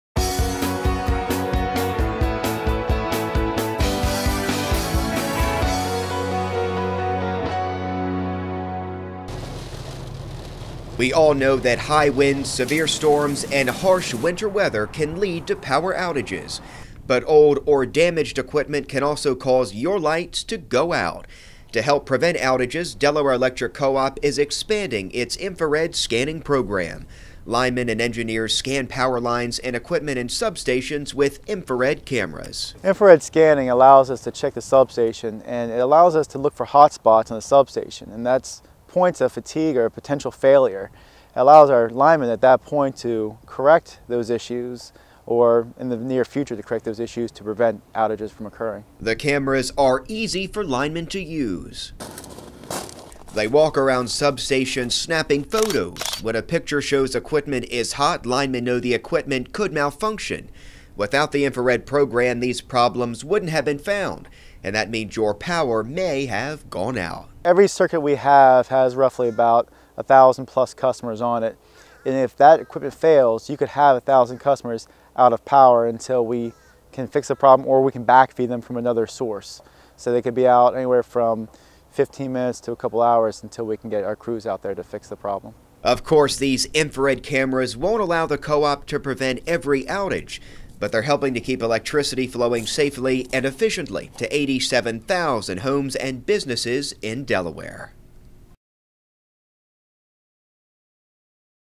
Behind the Scenes at DEC: Infrared Scanning:
We'll show you how linemen are using the cameras to find problems in substations.